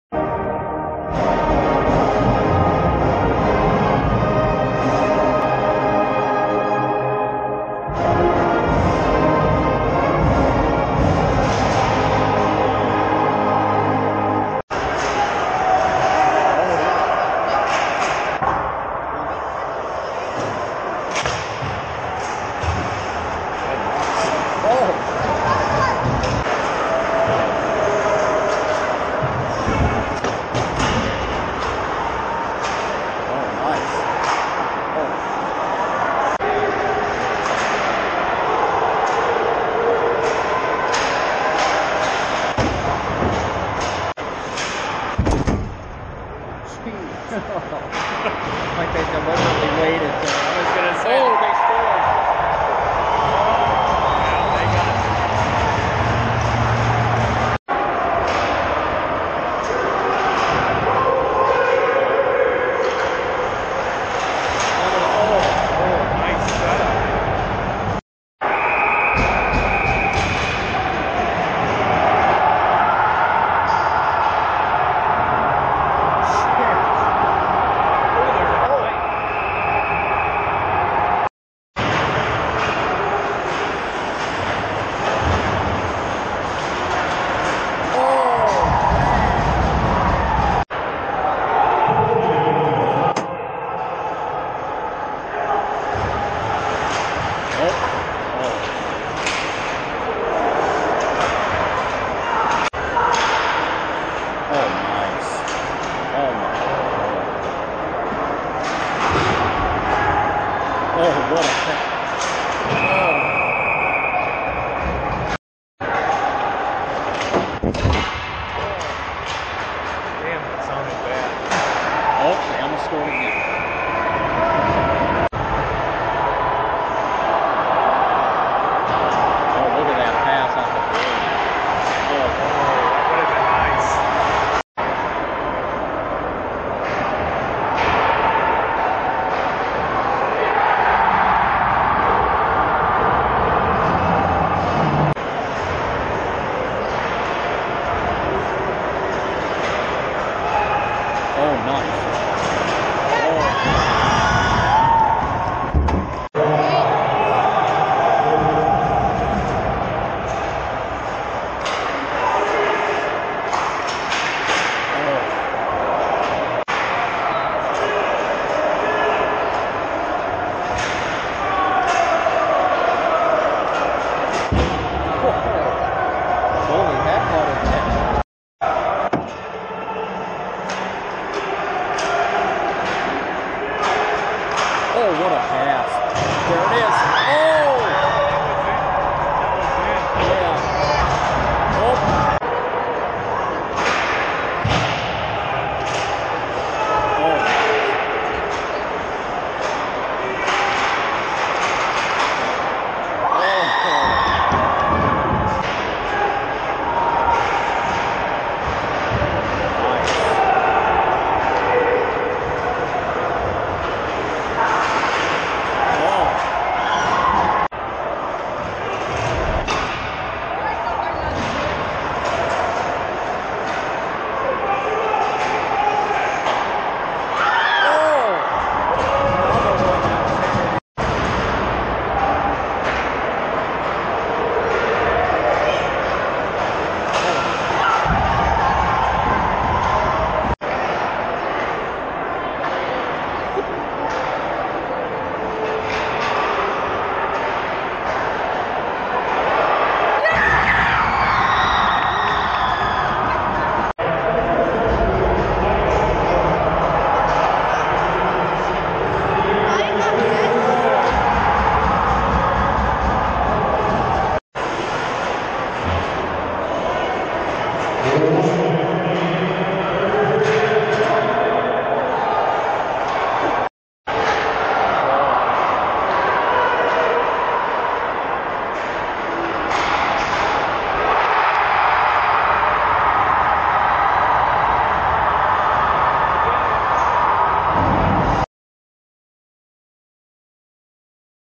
Hermantown @ Wayzata HIGHLIGHTS, 11/30/2018
WAYZATA defeats Hermantown in an OT thriller, 3-2 on November 30th, 2018 in Plymouth, MN